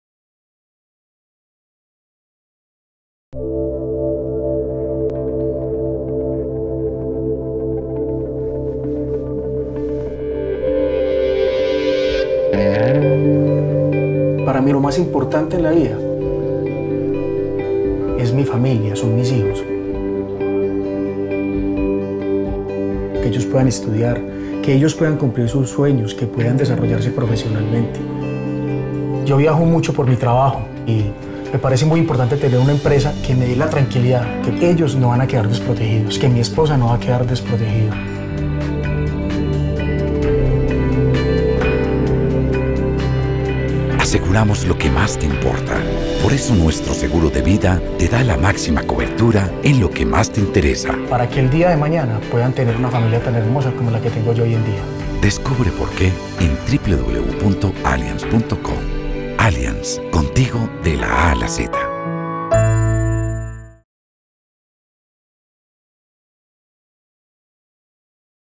Un buen futuro. Seguro de Vida Allianz.
Para Carlos Vélez lo más importante es el futuro de su familia.
En Allianz aseguramos lo que más te importa.